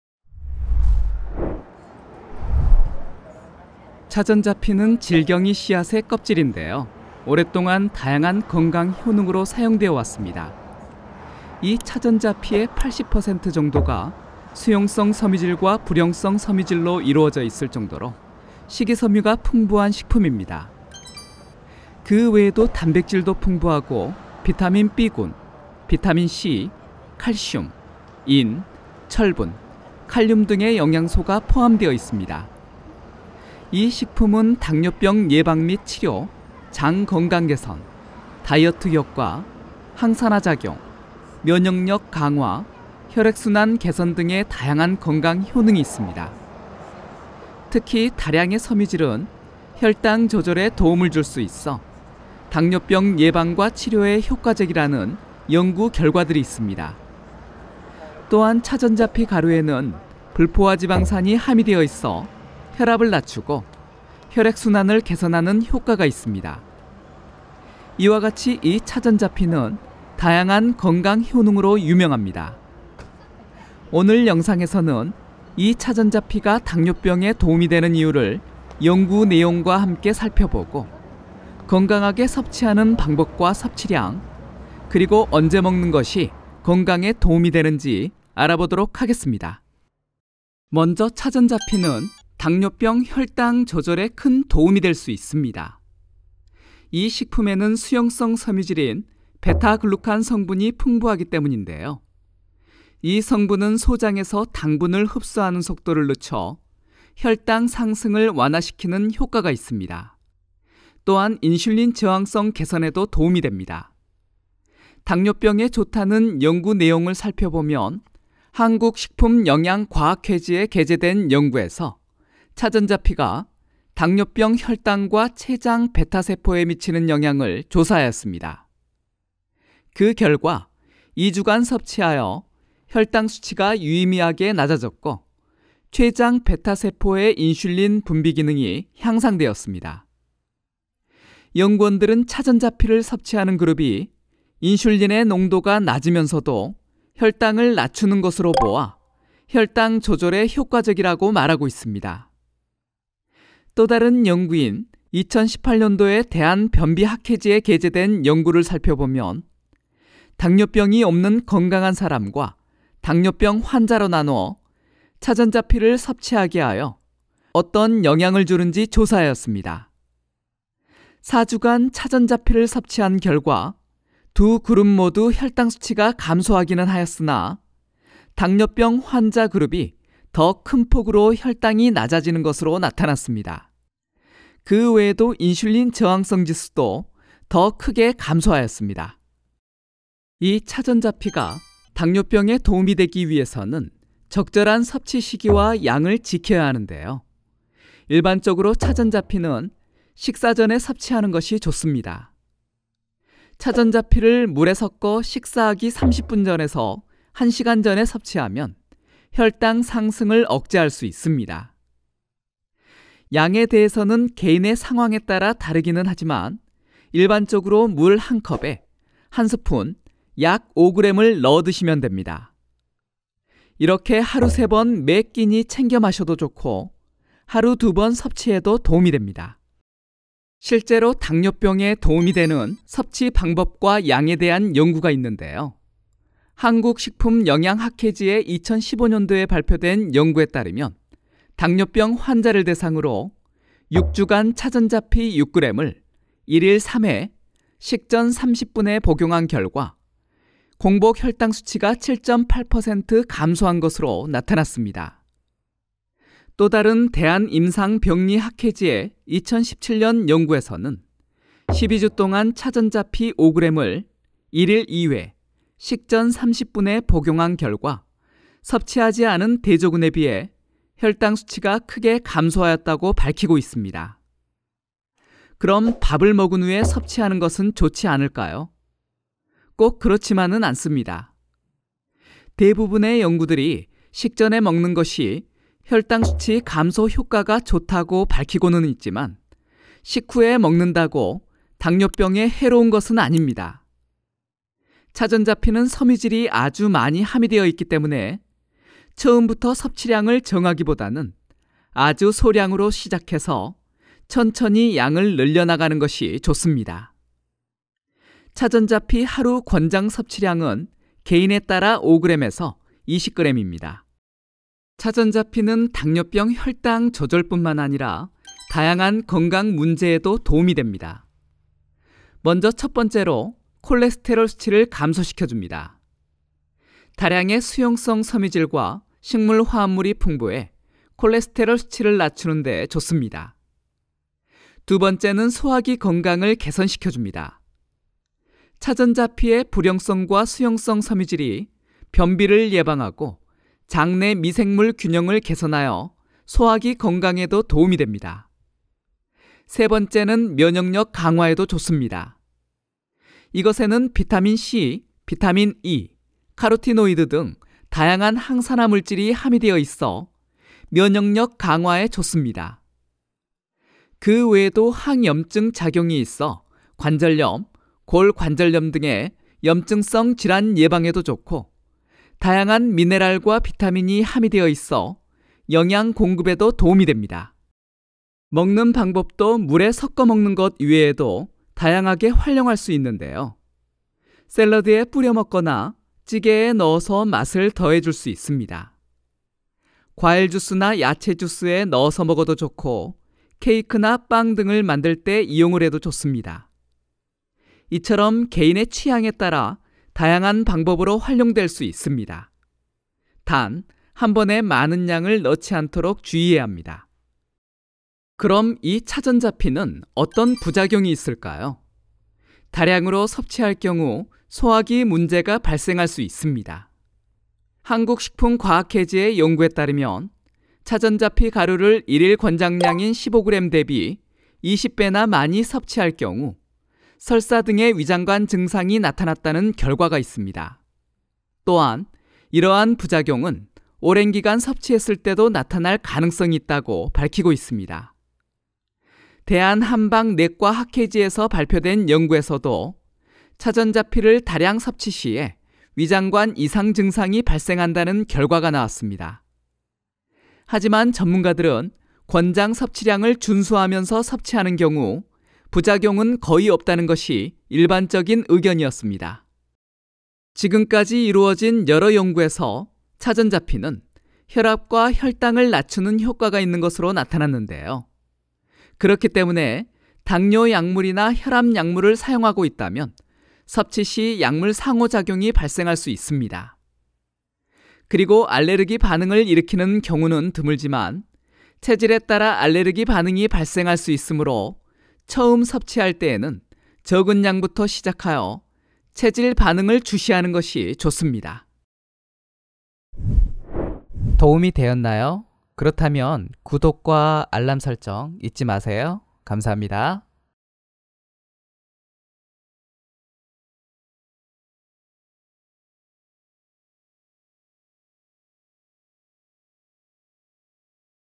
차전자피를 식전에 먹으면 당뇨병에 어떤 영향을 줄까요? 당뇨병에 좋은 차전자피!
당뇨병에 좋은 차전자피? 정말일까요? 당뇨병과 관련된 연구내용과 함께 차전자피의 효과를 알아보겠습니다.
한약재로 사용하는 차전자의 껍질이 바로 차전자피인데요! 이 식품이 건강에 좋다는 것이 잘 알려져 있습니다.
하지만 이 차전자피가 정말로 당뇨병에 도움이 될까요? 관련된 연구내용이 있습니다.
당뇨병에 좋은 차전자피로 많은 분들이 알고 계시는데요! 한국식품영양과학회지와 대한변비학회지에서도 당뇨병에 좋은 영향을 준다고 밝히고 있습니다.
그 내용과 당뇨병에 도움이 되는 먹는 시기와 양을 알아보겠습니다.